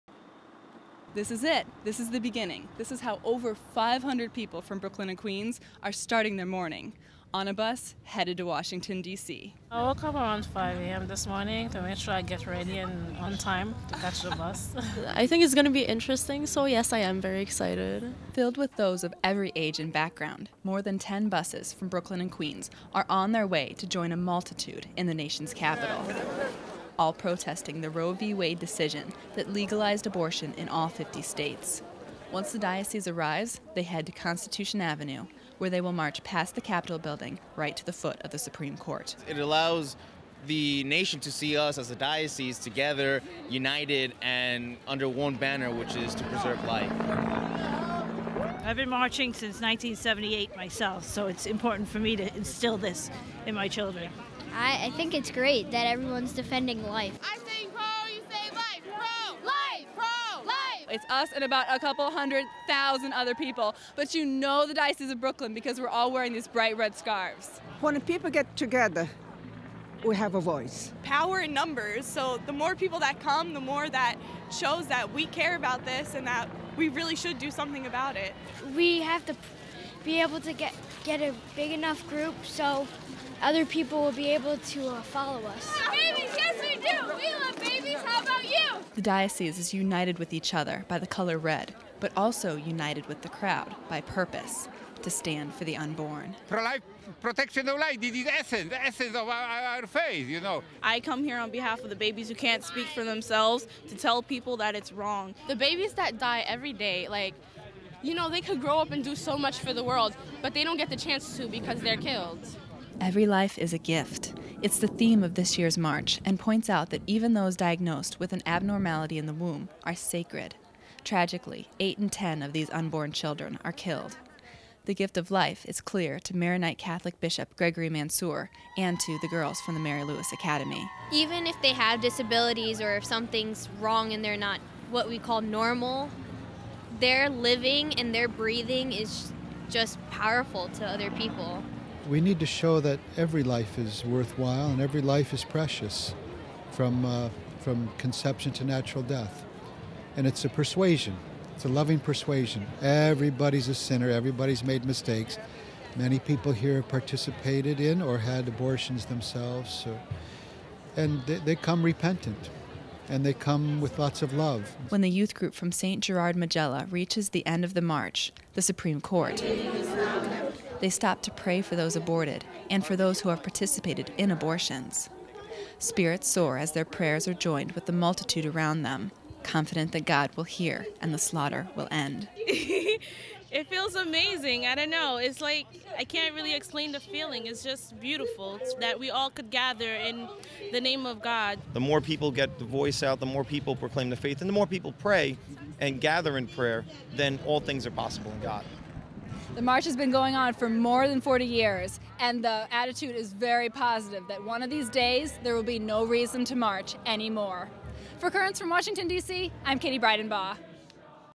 Brooklyn Diocese Represents at March for Life
Currents correspondent Katie Breidenbach reports on what the day was like for Brooklyn and Queens pilgrims at the March for Life.